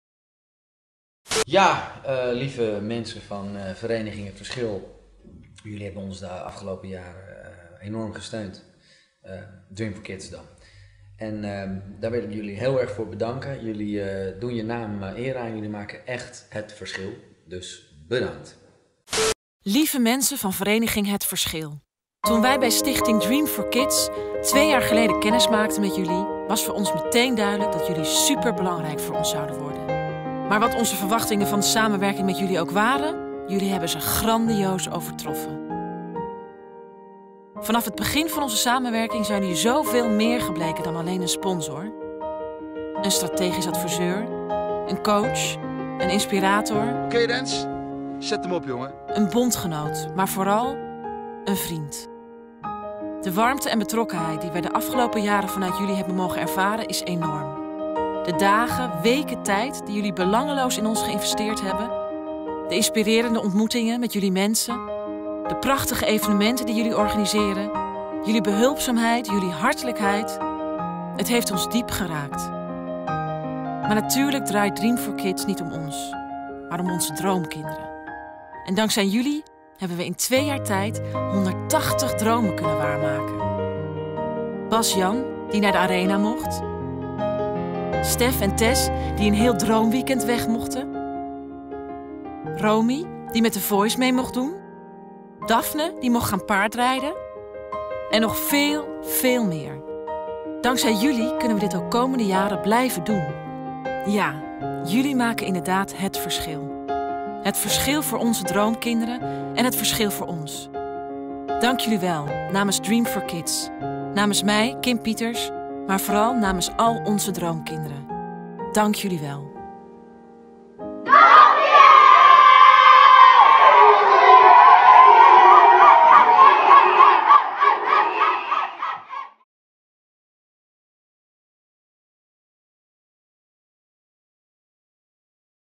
Dream4Kids bedankt Vereniging Het Verschil
Dream4Kids bedankt Vereniging Het Verschil voor de fijne samenwerking